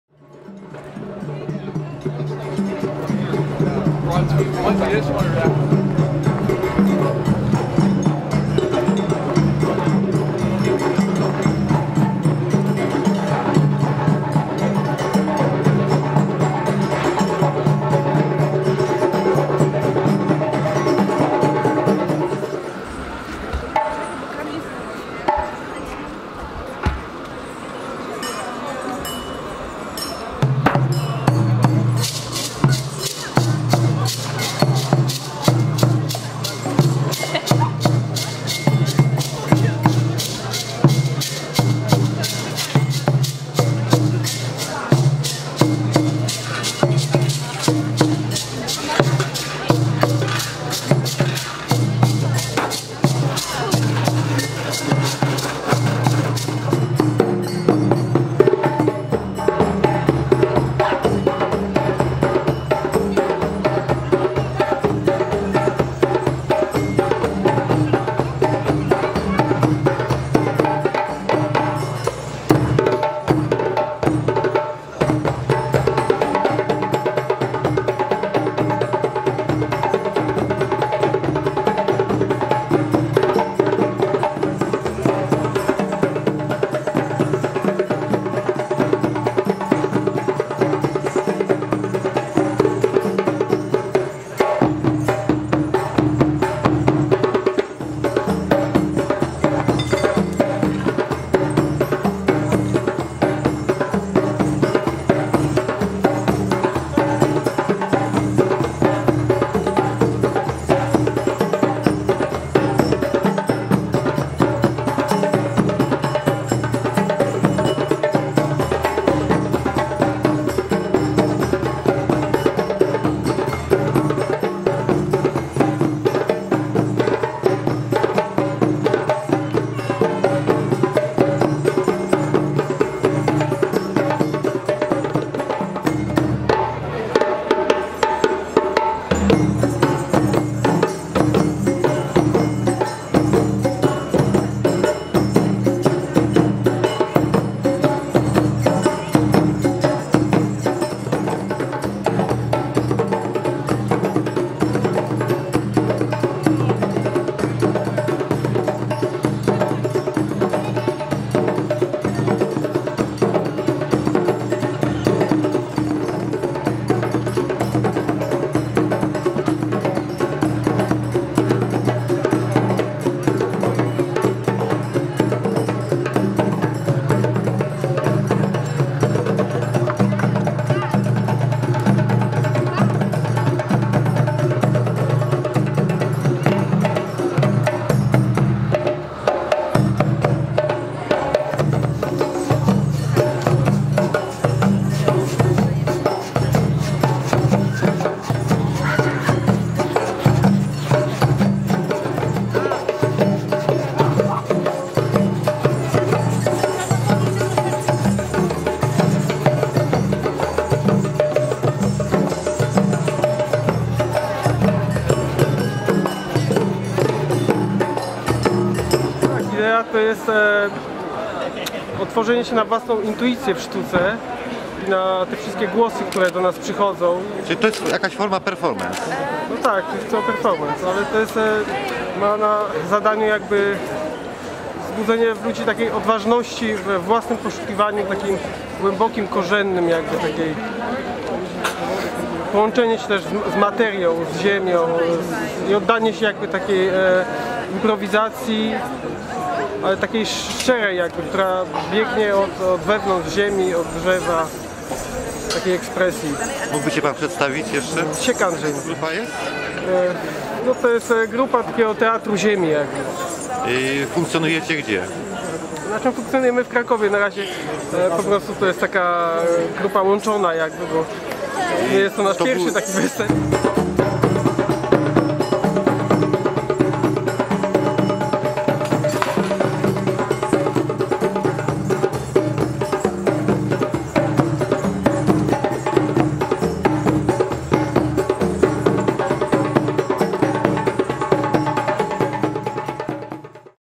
Grupa Teatru Ziemi
Czasami tak to jest, że pierwszoplanowe wydarzenia schodzą na drugi plan, w dniu 14 maja 2010 roku, tym pierwszoplanowym wydarzeniem była Noc Muzeów, kiedy przechodziłem przez Rynek Główny spostrzegłem niewielki tłumek otaczający twórców Grupy Teatru Ziemia, był ich to plenerowy debiut, myślę, iż następne spotkanie zaowocuje jeszcze większym materiałem filmowym, a twórcy będą pozyskiwać coraz większą grupę fanów.
Pozdrawiam ich wszystkich.
F0409